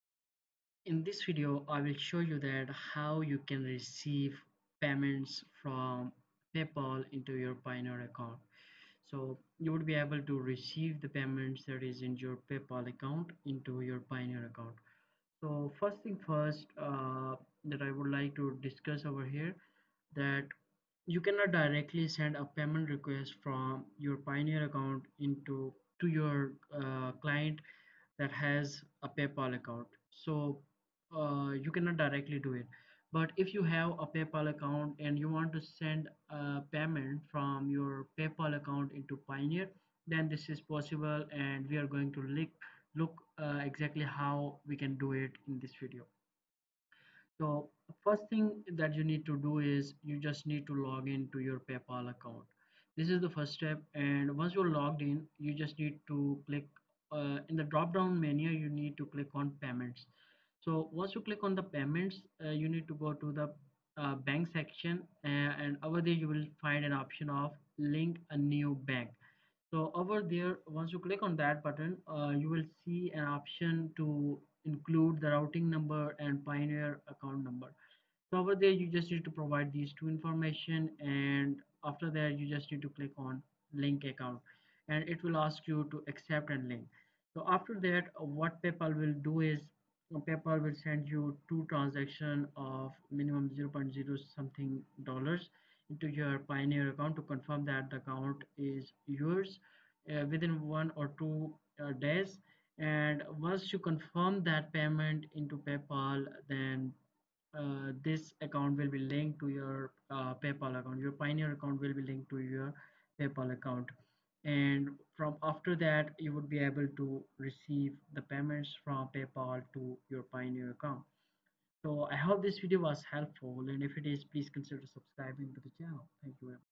HOW TO SEND PAYMENT REQUEST ON PAYONEER TO PAYPAL (Full Guide)
HOW TO SEND PAYMENT REQUEST ON PAYONEER TO PAYPAL (2024 UPDATED)

In this video, I'll guide you through the steps to request a payment from a PayPal user to your Payoneer account. Follow this tutorial to manage your funds efficiently between the two platforms.

Timestamp:
00:00 Intro
00:09 SEND Payment Request on Payoneer to PayPal
01:55 Outro